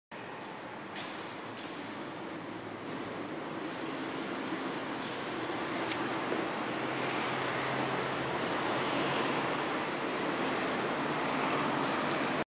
埼玉県さいたま市 貸し倉庫 格安 新着物件 ビーズ
さいたま市桜区新開に330坪の2階建て貸し倉庫あります。綺麗☆事務所付☆大型車可☆一階部分の動画です。
さいたま市の賃料が安いお得な貸し倉庫です。